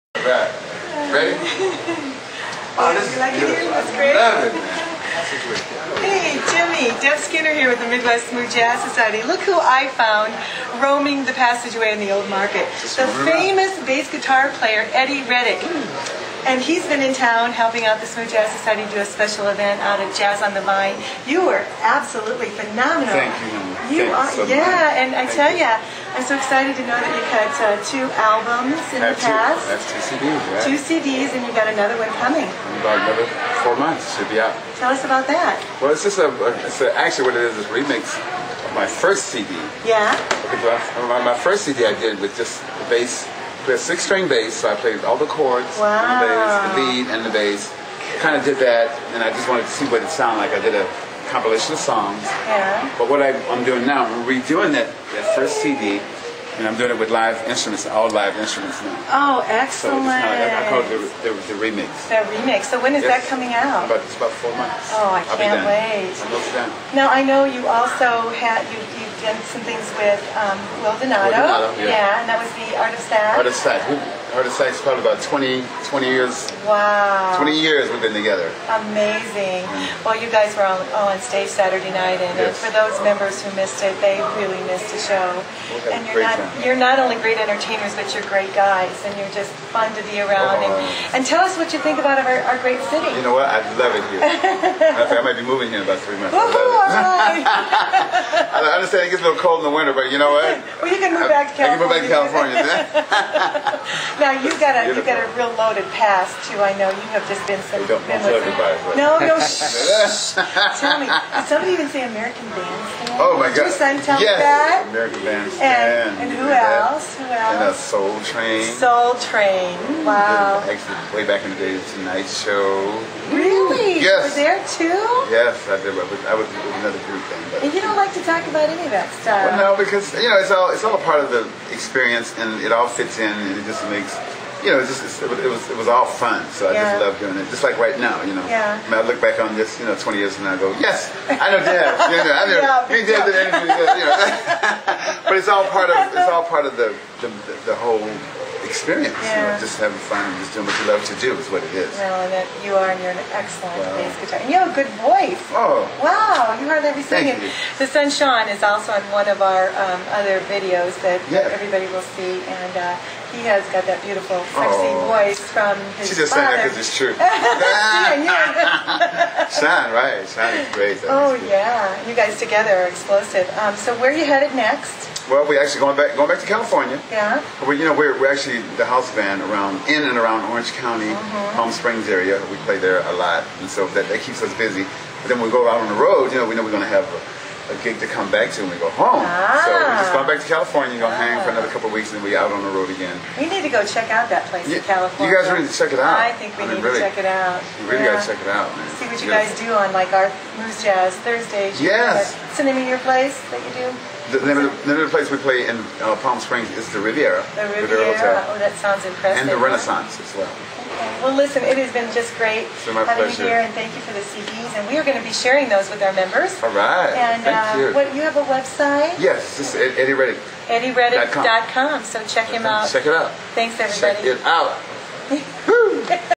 Eddie Reddick Interview
Deb Skinner President of the Midwest Smooth Jazz Society interviews Eddie Reddick bassist for The Art Of Sax. Eddie joined Will Donato, Johnny Meza, Shon Reddick Blake Aaron and Pat Ready on stage at Jazz On The Vine concert. All brought to Springfield NE. by Innervision Radion and the Midwest Smoo